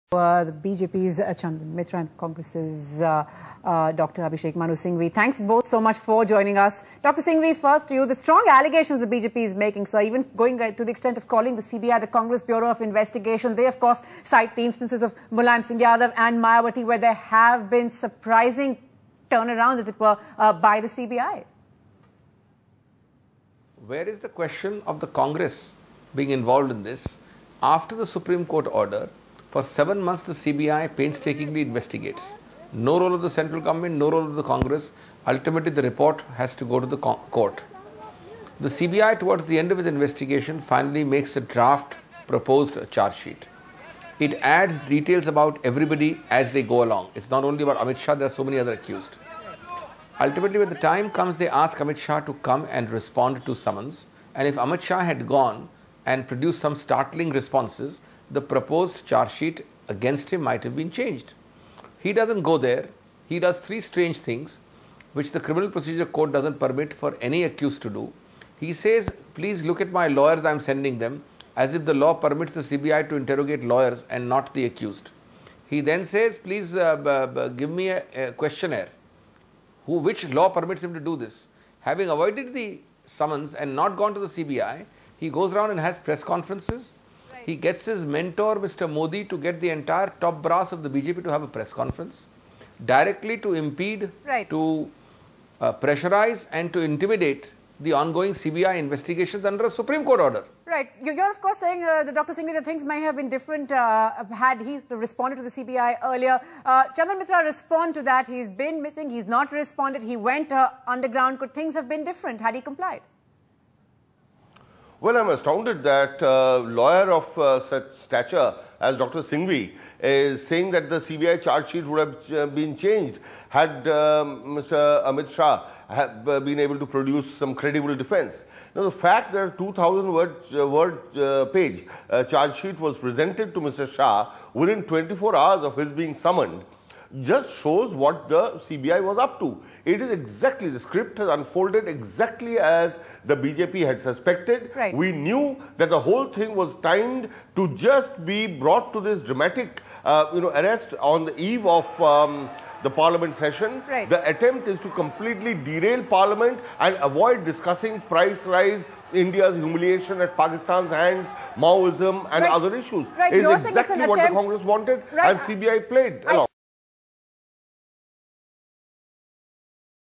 Congress: Let court decide who's guilty
Congress spokesperson Abhishek Manu Singhvi refuted BJP's allegation that the Center was misusing the CBI in Amit Shah case. He said that the CBI was asked to investigate by the Supreme Court and that Congress party has role in it.